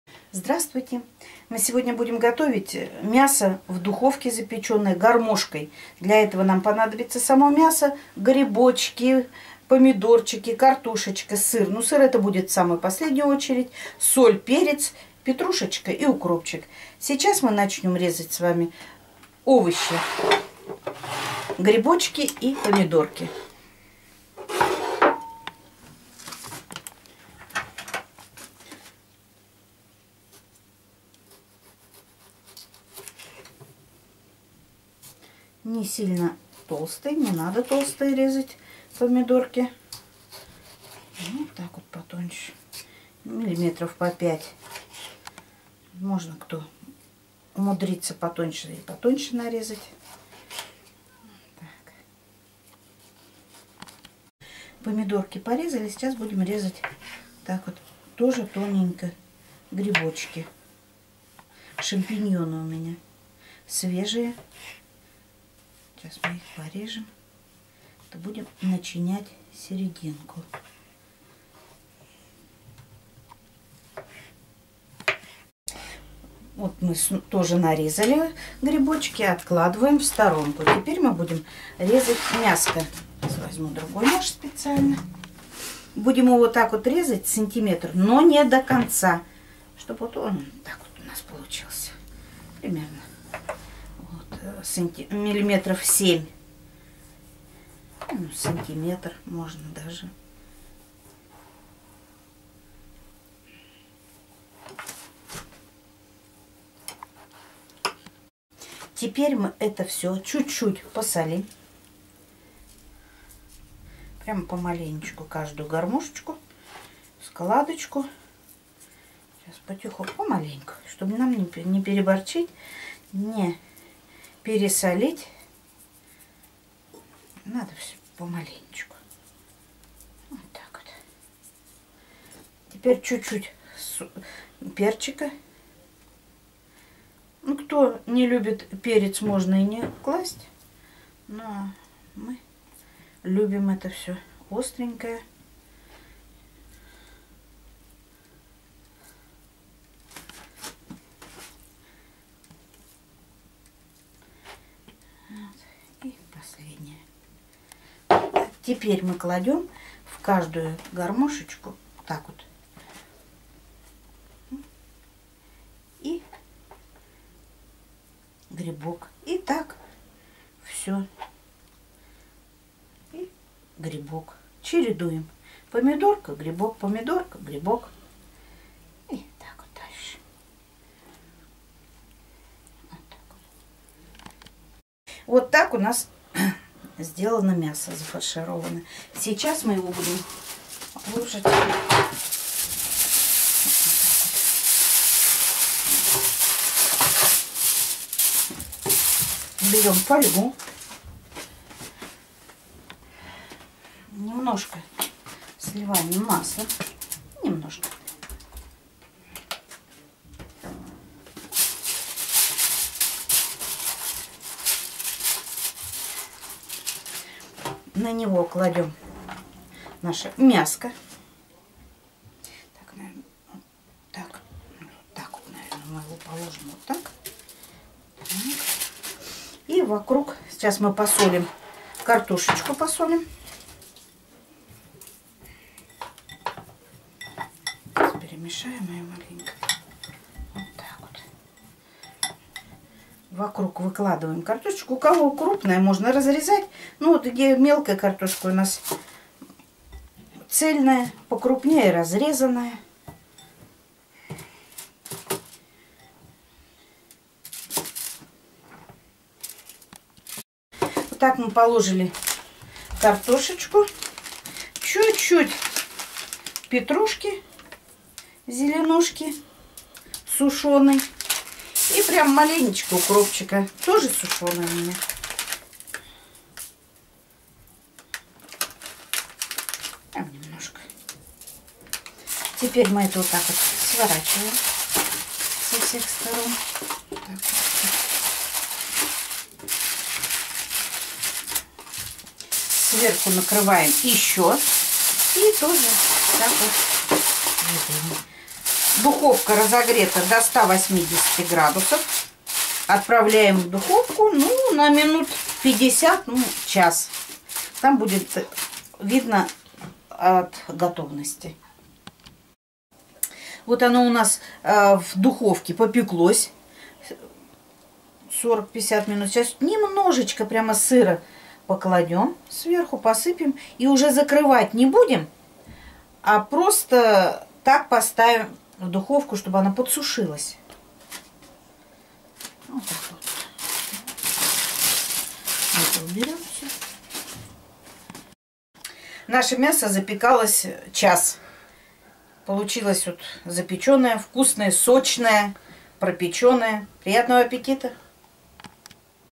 Мясо с картошкой в духовке / Горячее блюдо на праздничный стол.
Мясо с картошкой в духовке - это красивое и вкусное блюдо.  Запекать мясо с картофелем будем в духовке без майонеза. Мясо получается вкусное и нежное. Это одно из лучших мясных блюд для праздничного стола.
Я покажу свой рецепт мяса с картошкой в духовке на праздничный стол..

Для того, чтобы вкусно запечь мясо в духовке на праздничный стол:

    Мясо - 1 кг. ( я взяла мякоть свинины. Вы можете взять любое другое мясо. )
    Шампиньоны 6 штук.
    Картофель 4-5 штук.

Как вкусно приготовить мясо в духовке:

    1. Первым делом необходимо нарезать мясо. В куске свинины сделать надрезы (не доходя до конца), толщиной около 1 см.  Мясо со всех сторон и все складочки необходимо посолить и поперчить.
    2. Теперь нарезаем овощи. Шампиньоны нарезаем дольками. Нарезаем кружочками помидоры.
    3. Овощи подготовлены, будем фаршировать мясо. В разрез выкладываем кружочек помидора и дольку шампиньона. И так в каждый мясной разрез.
    4. Переносим фаршированное мясо на лист фольги, который предварительно нужно смазать растительным маслом.
    5. Картофель разрезаем на небольшие кусочки в длину. Присаливаем. Выкладываем картофель вокруг мяса по периметру.
    6. Заворачиваем края фольги. Сверху накрываем тоже фольгой. Отправляем противень с мясом в духовку, разогретую на 180 градусов, запекаться на 60 минут.
    7. Через 50 минут вынимаем мясо из духовки. Снимаем верхний лист фольги, посыпаем тертым сыром и отправляем мясо в духовку  запекаться до золотистой корочки.
         Прошло 10 минут и мясо готово.

Вот так просто можно приготовить мясо в духовке с овощами.

Рекомендую посмотреть МОИ ПЛЕЙЛИСТЫ:
2. Рецепты заготовок на зиму
8. Рецепты разных видов теста